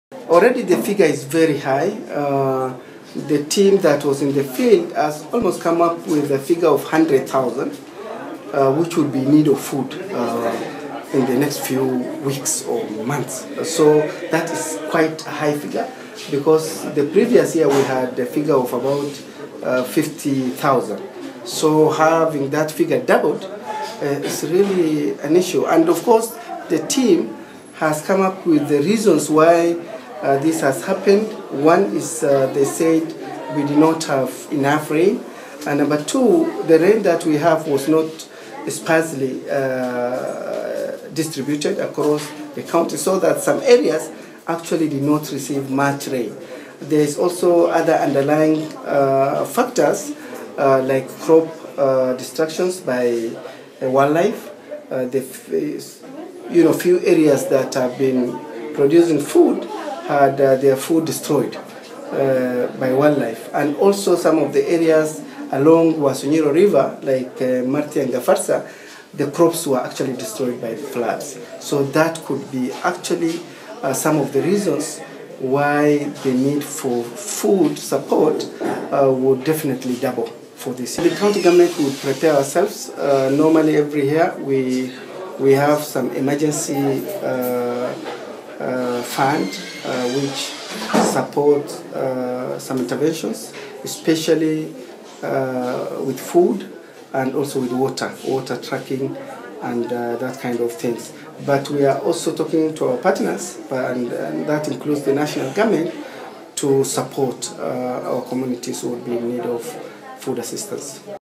About 100,000 people in Isiolo County are in need of food aid
Isiolo governor said that the county is in need of food aid and about 100,000 people are affected.